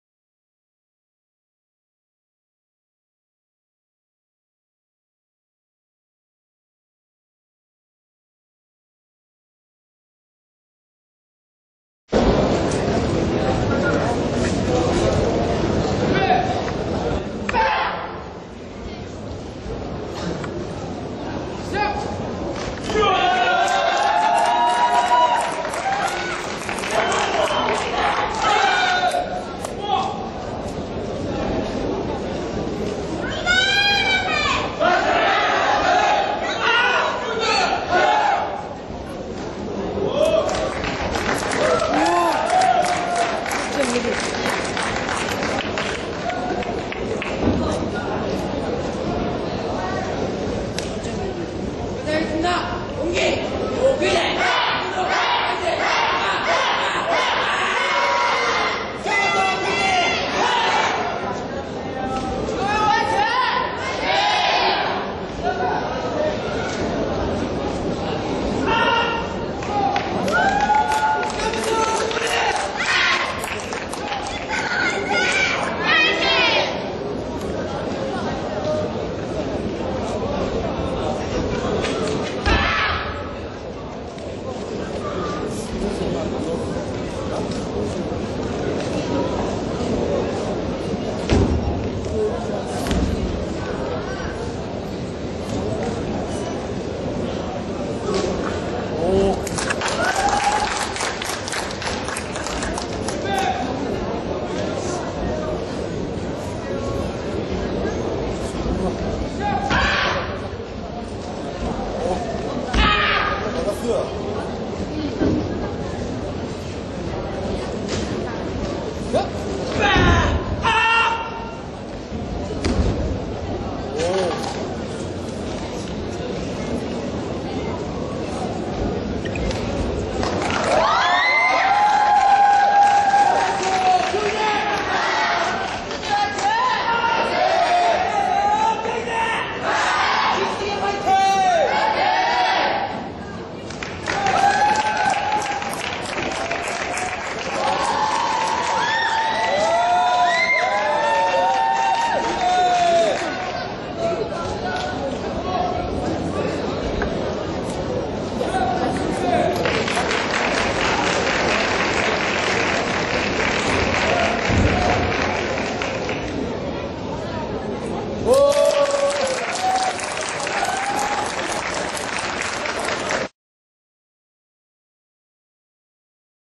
제10회우석대총장기 남대1부1조 신태범(동아대)vs정기현(우석대)
Taekwondo_Poomsae
2012년 7월13일~15일/3일간